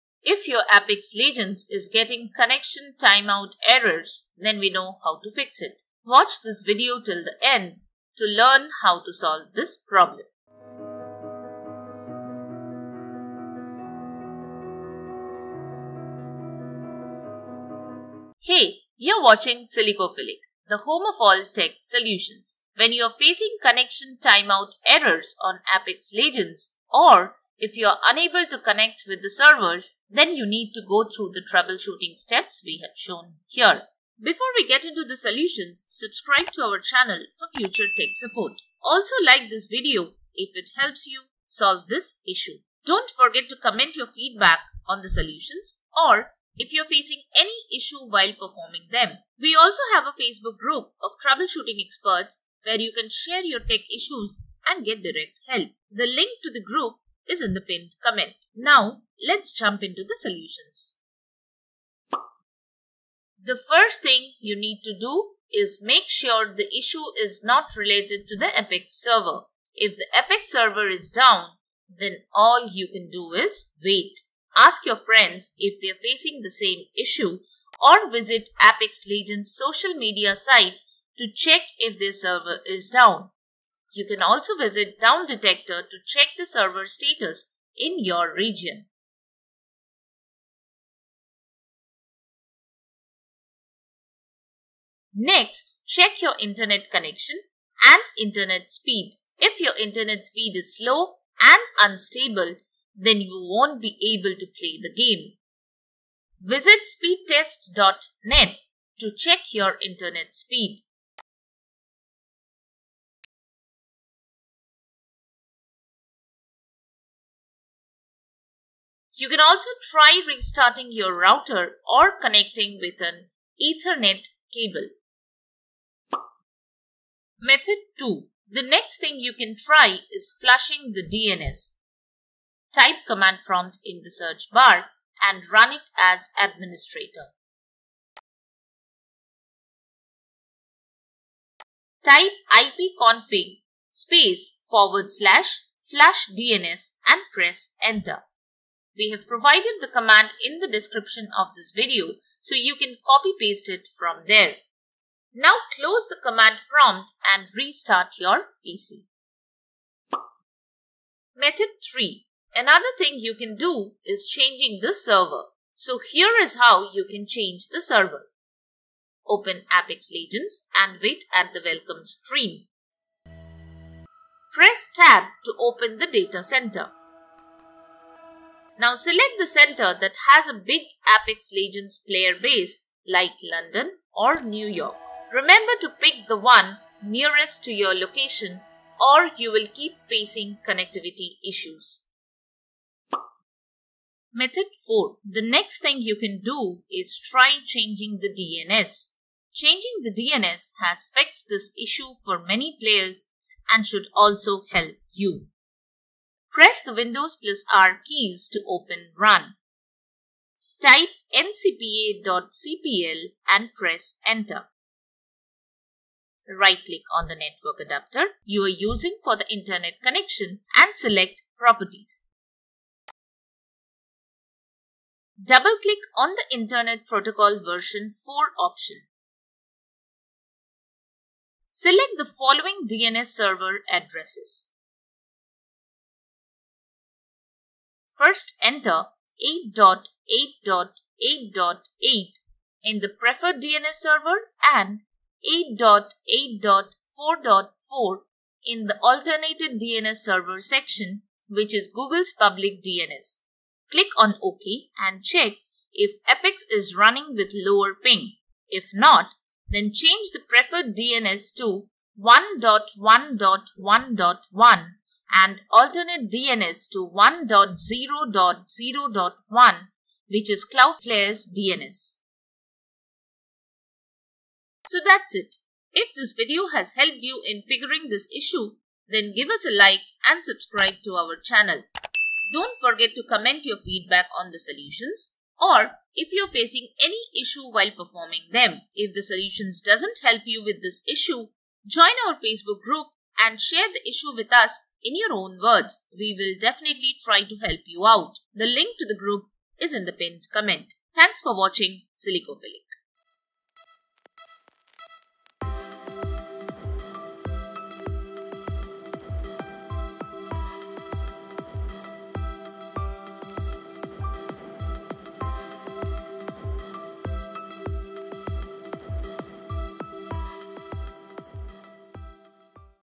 How To Fix Apex Legends Connection Timed Out? [Working Methods]
If your Apex Legends is getting connection time out errors, then we know how to fix it. Watch this video till the end to learn how to solve this problem.

Command Used:
ipconfig /flushdns

Timestamps:
00:00 Intro
01:02 Check Internet Connection
02:06 Flush The DNS
02:40 Change Server
03:13 Change The DNS